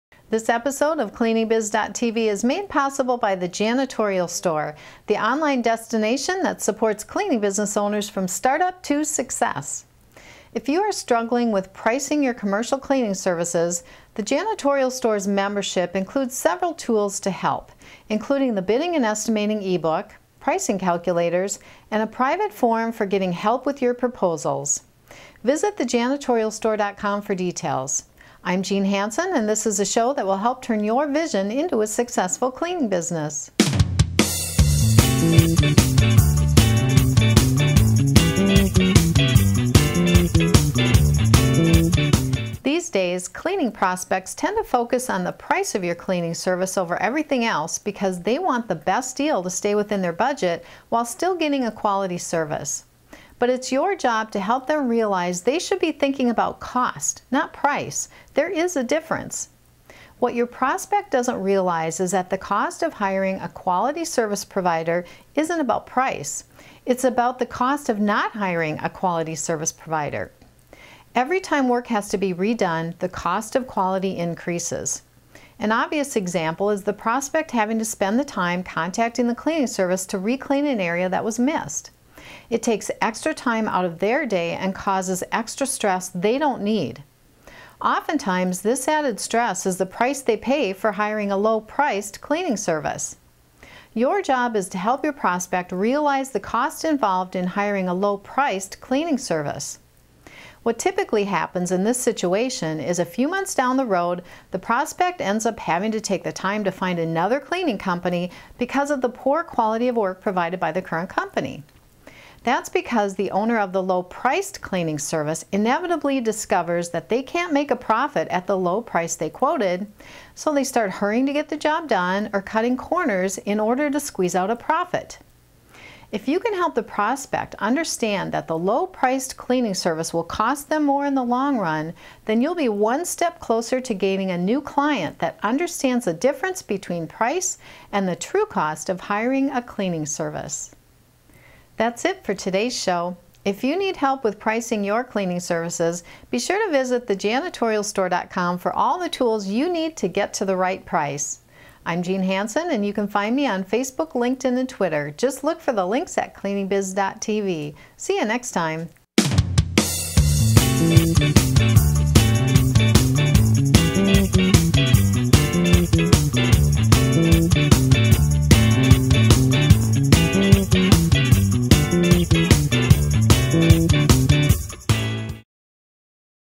Price Versus Cost: There IS a Difference When it Comes to Selling Cleaning Services
- These days cleaning prospects tend to focus on the price of your cleaning service over everything else because they want the best deal to stay within their budget, while still getting a quality service. But it's your job to help them realize they should be thinking about COST, not PRICE - there IS a difference. Watch the video to learn just what that difference is, and then post your comments below.